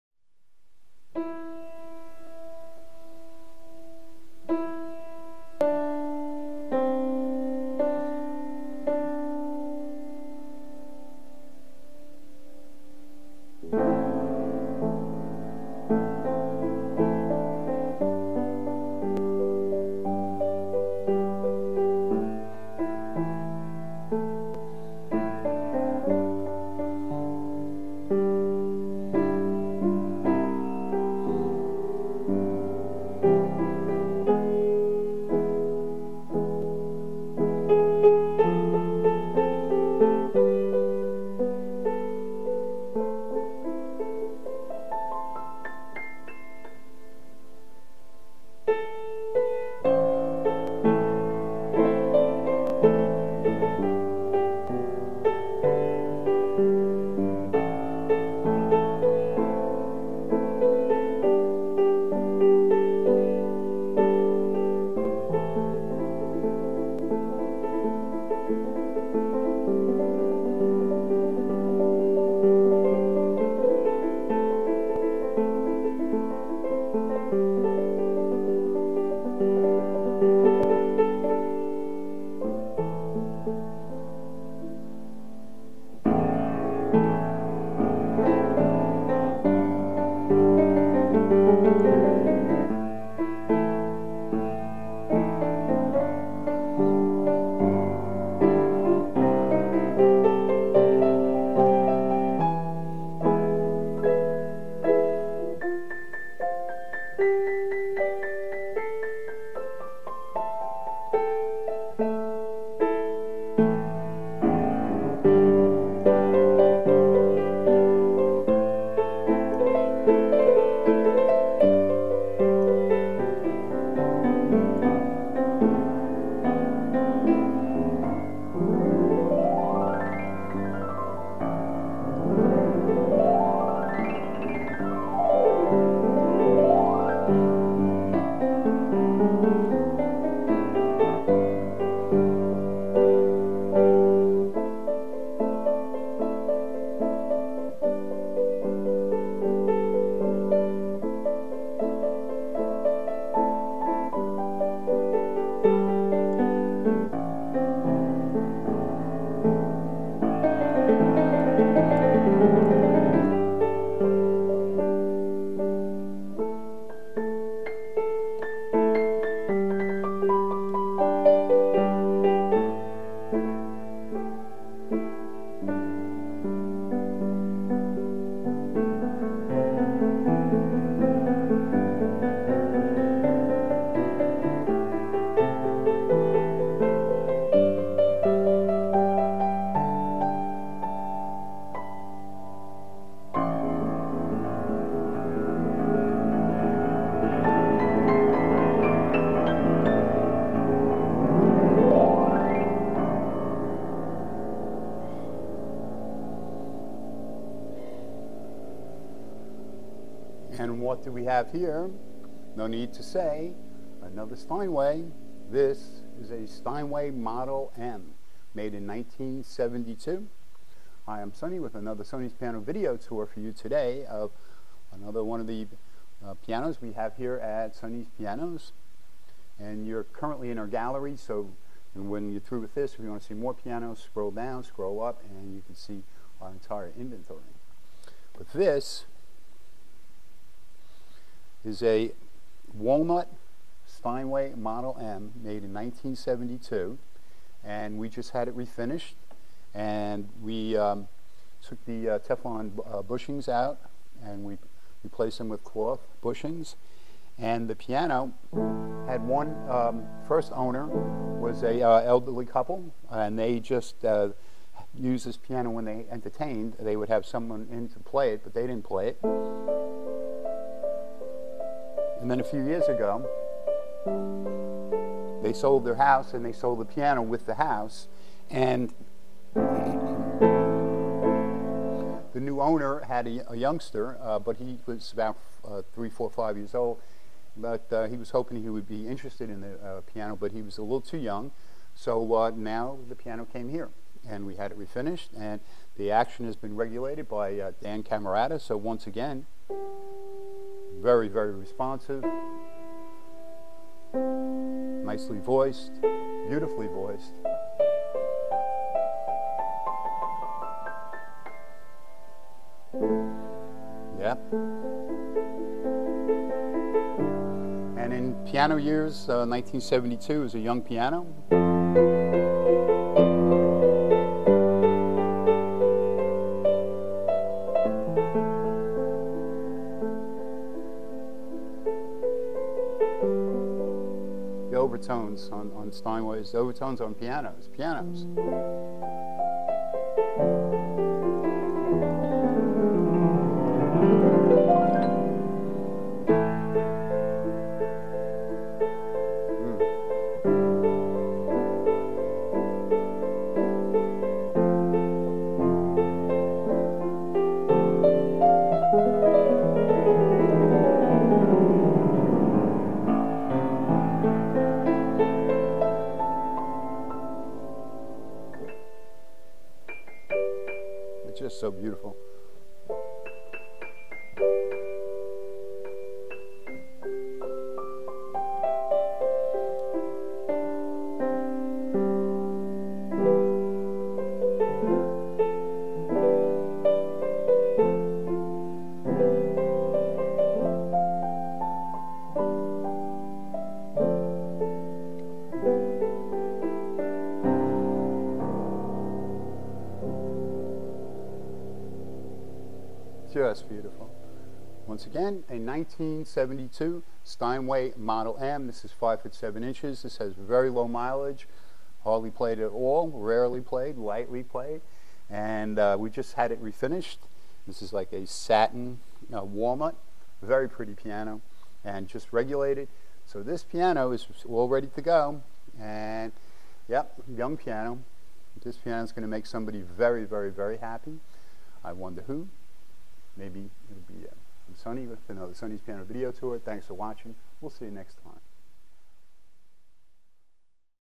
Steinway Grand Piano Model M 1972 Walnut Refinished/Refurbished $17,500.
See more video tours of pianos for sale here
Steinway Grand Piano Model M, 1972, beautiful satin walnut, just back from the refinishers, action just regulated., all original Steinway parts in excellent condition. No teflon bushings in hammers and shanks, just replaced with cloth bushings. Sublime tone, great action response. Only 38 y/o young. Two owners, piano saw very little playing time. New M's sell for around $55K. Free LI/NYC delivery all else continental USA only $475.Warranty. Questions? Call Sonny Direct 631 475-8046.